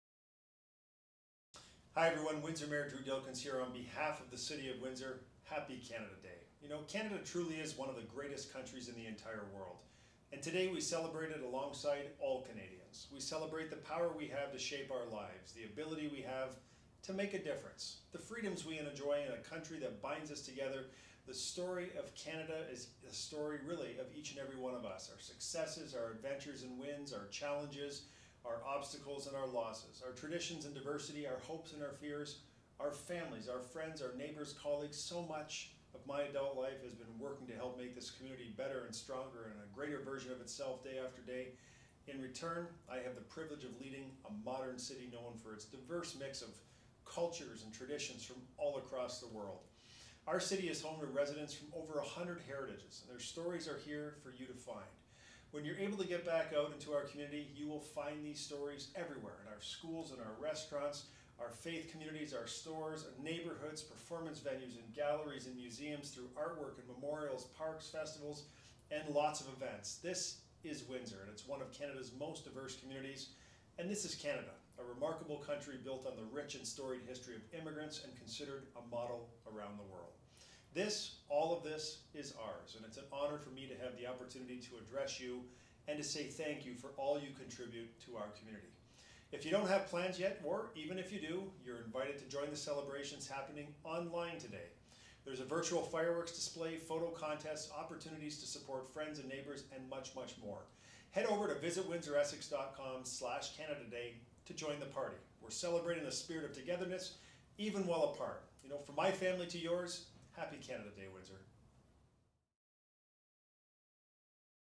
Happy Canada Day 2020
Happy Canada Day, Windsor! Our festivities look very different this year, but we have no less reason to celebrate this incredible country of opportunity, freedom, diversity, and inclusion. From my family to yours, Happy Canada Day.